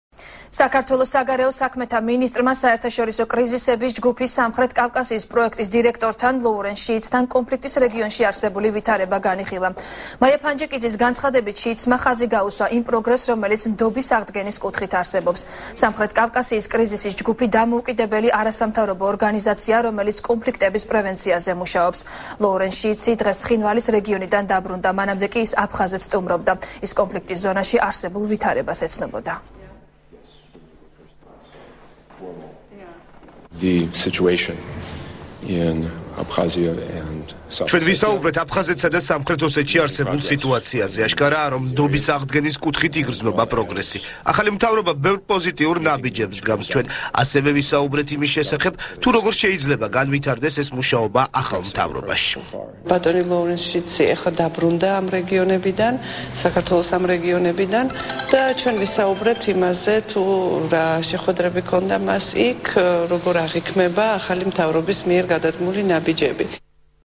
ახალი 3 | ფანჯიკიძის შეხვედრა | 22.02.13
საქართველოს  საგარეო  საქმეთა  მინისტრმა  მაია ფანჯიკიძემ საერთაშორისო კრიზისების ჯგუფის სამხრეთ კავკასიის პროექტის დირექტორთან ლოურენს შიითსთან კონფლიქტის რეგიონში არსებული ვითარება განიხილა. ფანჯიკიძის განცხადებით, შიითსმა ხაზი გაუსვა იმ პროგრესს, რომელიც ნდობის აღდგენის კუთხით  არსებობ". სამხრეთ კავკასიის კრიზისის ჯგუფი დამოუკიდებელი არასამთავრობო ორგანიზაციაა, რომელიც კონფლიქტების პრევენციაზე მუშაობს. ლოურენს შიითსი დღეს ცხინვალის რეგიონიდან დაბრუნდა. მანამდე კი აფხაზეთს სტუმრობდა. ის კონფლიქტის ზონაში არსებულ ვითარებას ეცნობოდა.